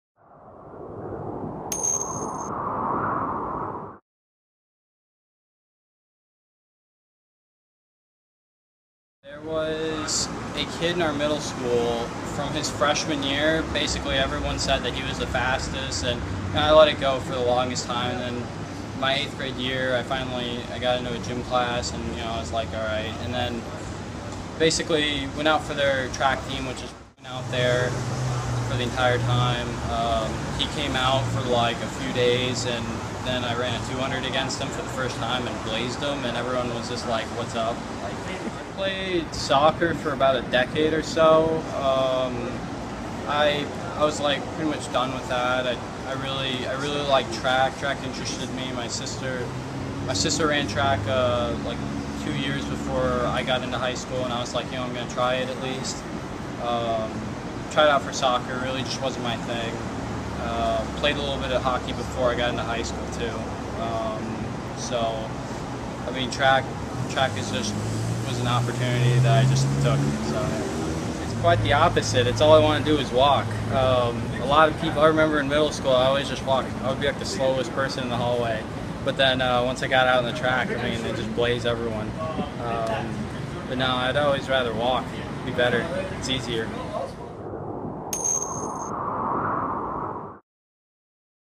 HT PREPS: Charlotte's Devin Quinn sprints to the Runner of the Year honor
A Class 3A state champ in the 100-meter run, Charlotte's Devin Quinn is the All-Area Boys Runner of the Year.